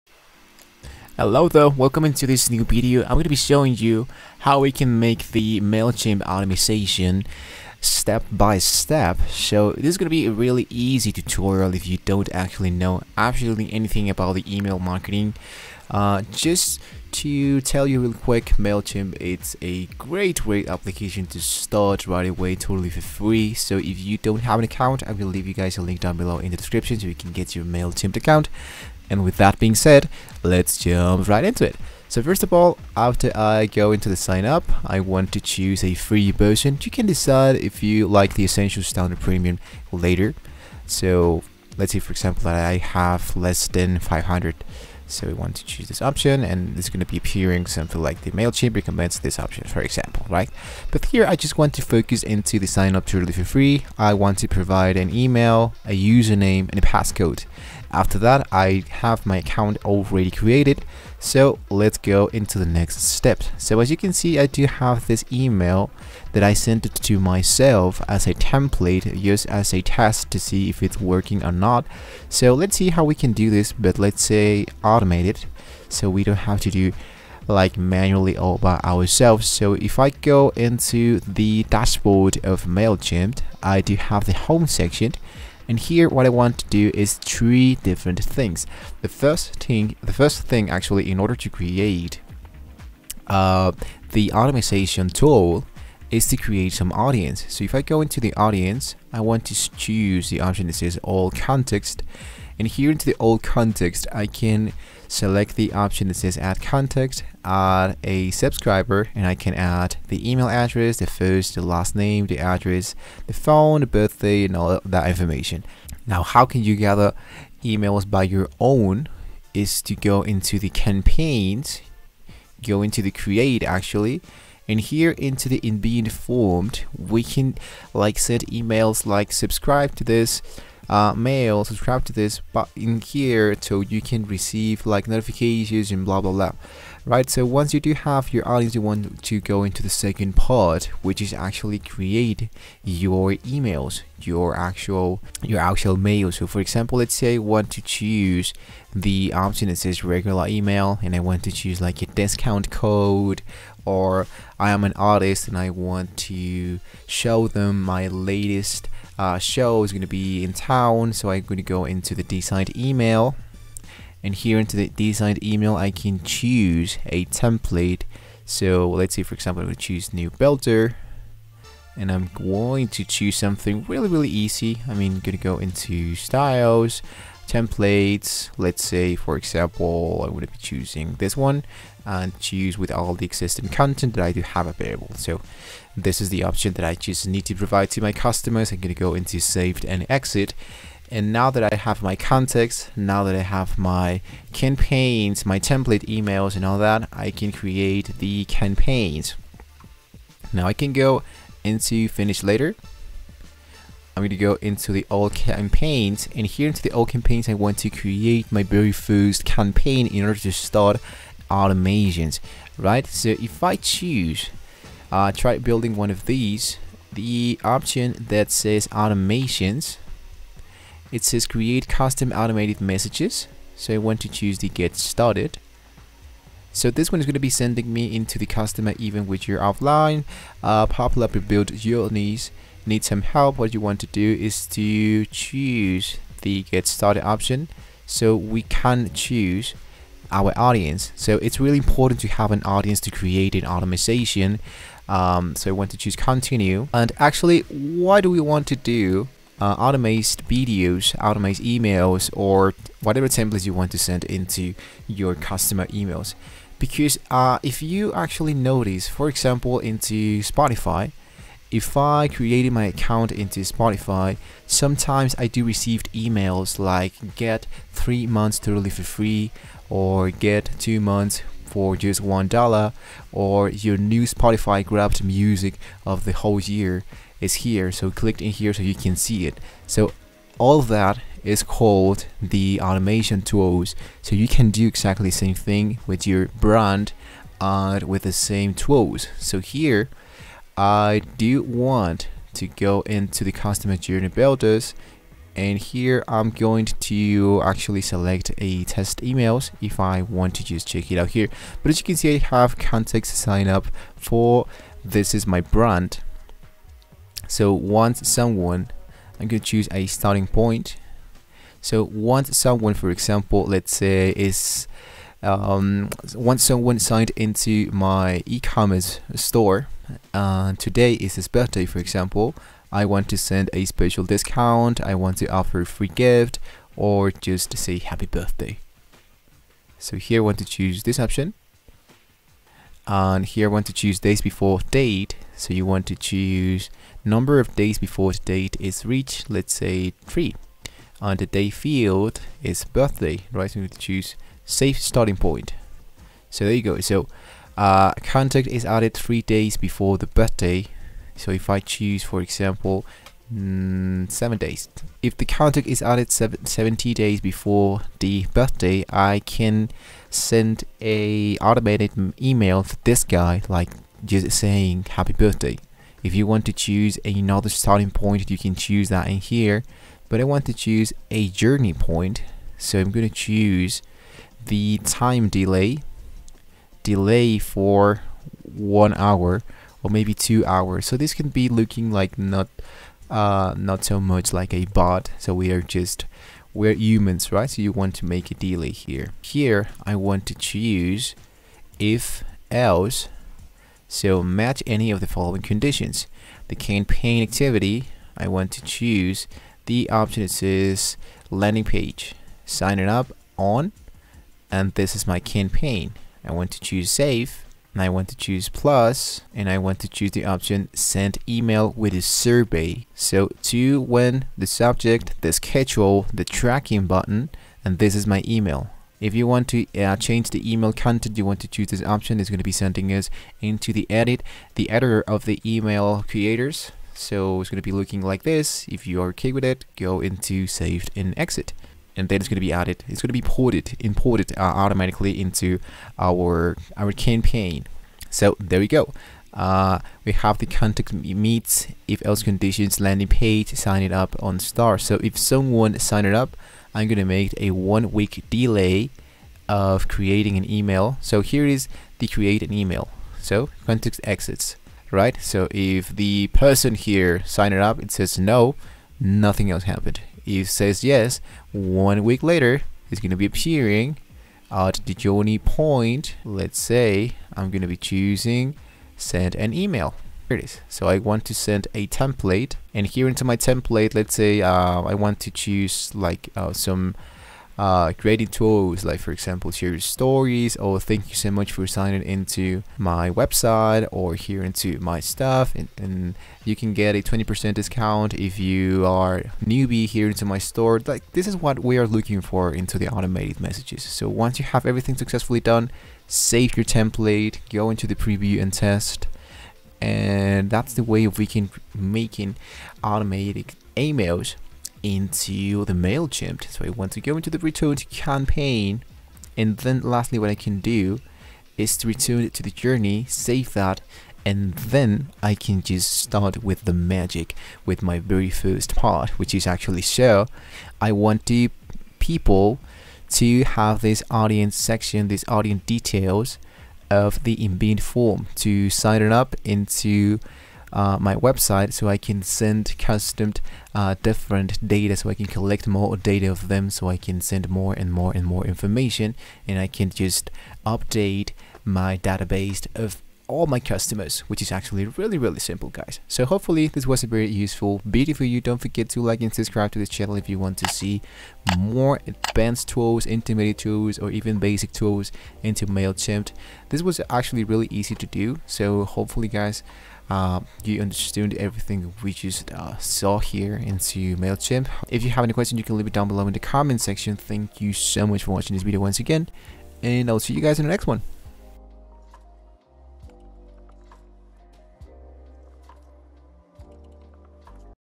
Mailchimp Automation Sequence For Beginners (2024) Complete Mailchimp Guide
MailChimp Automation Sequence for Beginners is an essential tool for any business. Through MailChimp, businesses are able to create automated campaigns that target specific groups of customers, allowing them to save time while still providing an engaging experience to their customers.

The process involves setting up an automated sequence of emails that activate when a specific action is triggered. This could be triggered by an event, like a customer buying a product, signing up for a newsletter, or completing a survey.

By setting up a sequence of emails, businesses can control the flow of information and customize it to target specific audiences. Additionally, using MailChimp’s analytics tools, businesses can track the performance of their automated campaigns, making adjustments to optimize their campaigns. MailChimp automation sequences for beginners are a powerful tool to have in any marketer’s arsenal.